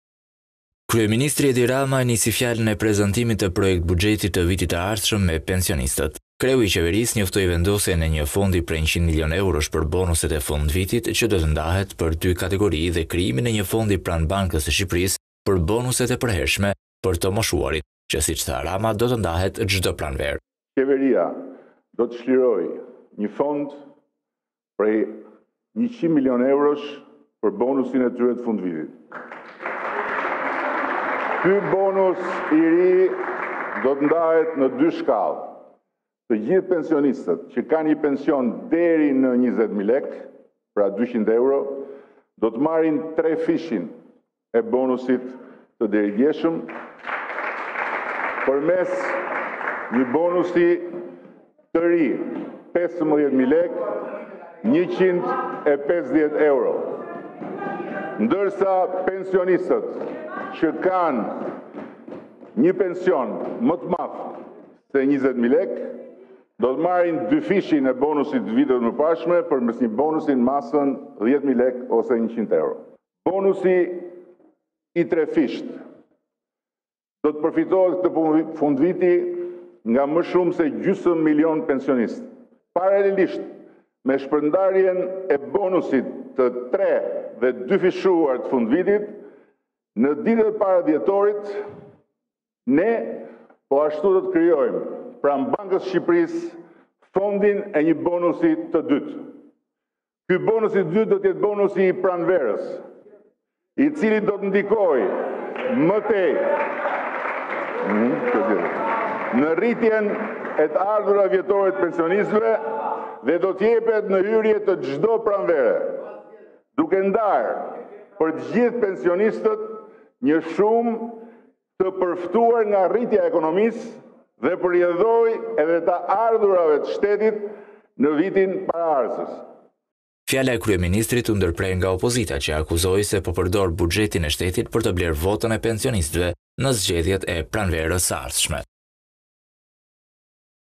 Top News/ Trefishohet bonusi për pensionistët. Qeveria ndan 100 mln euro për të moshuarit
Kryeministri Edi Rama e nisi fjalën e prezantimit të projektbuxhetit të vitit të ardhshëm me pensionistët. Kreu i Qeverisë njoftoi vendosjen e një fondi prej 100 milionë eurosh për bonuset e fundvitit, që do ndahet për dy kategori dhe krijimin e një fondi pranë Bankës së Shqipërisë për bonuse të përhershme për të moshuarit, që sicc tha Rama, do të ndahet ccdo pranverë.

Fjala e Kryeministrit u ndërpre nga opozita, që e akuzoi se po përdor buxhetin e shtetit për të blerë votën e pensionistëve në zgjedhjet e pranverës së ardhshme.